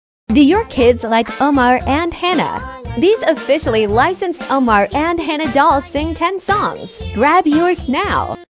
Omar and Hana just got better with these officially licensed Omar and Hana singing dolls
Omar and Hana just got better with these officially licensed Omar and Hana singing dolls. Not only do they sing 10 songs but they’re also cute and cuddly. The perfect buddy for the summer holidays.